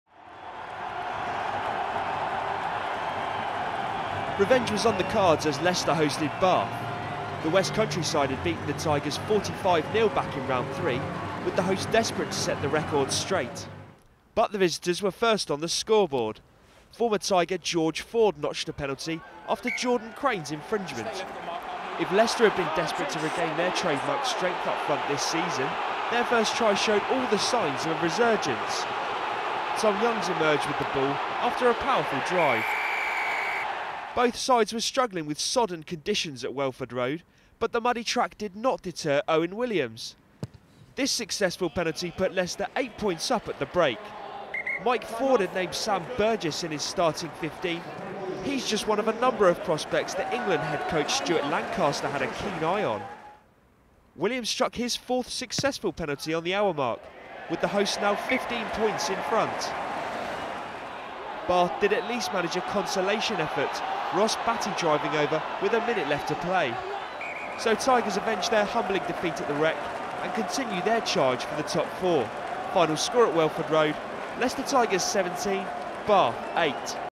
Leicester Tigers v Bath - Aviva Premiership Rugby 2014/15
Highlights from the game at Welford Road where Leicester hosted Bath.